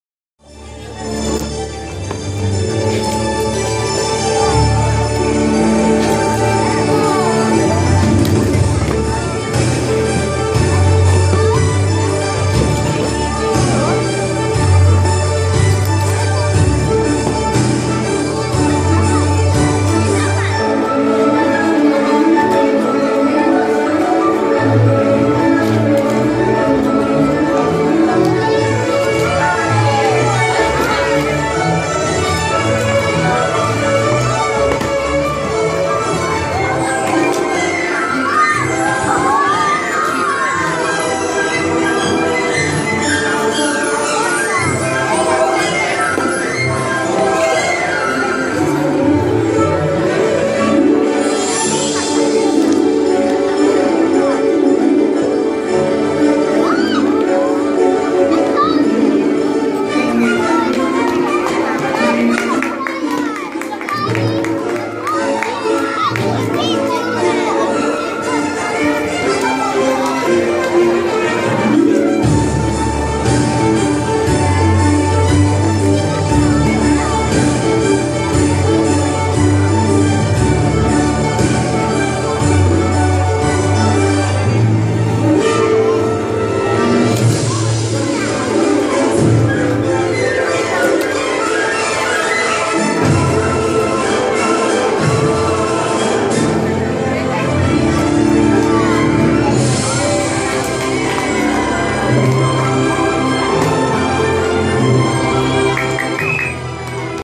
Kiddie party host magician Rjhay Clemente
One of my illusions during kiddie party gigs